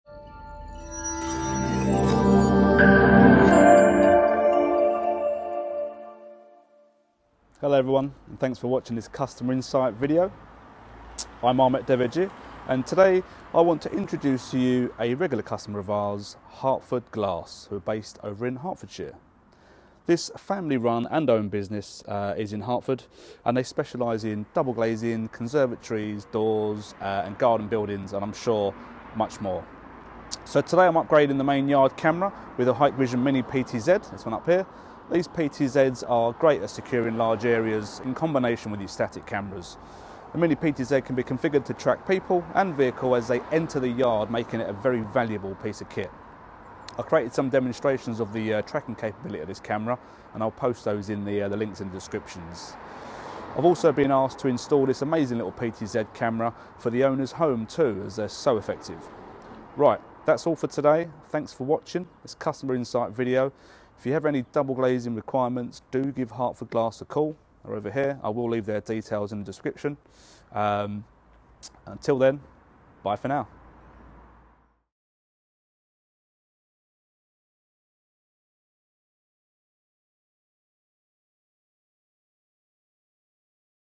Customer Insight - Hertford Glass
Today's customer insight video is Hertford Glass!

Hertford Glass was founded in 1971, to offer a glazing service for the local area.

The original founders cut precision glass for installation into single glazed windows, and this art is still practised by our skilled craftsmen today.

Hertford Glass is the trading name of Home Glazing Improvements

Camera model in use: HIKVISION DS-2DE4A425IW-DE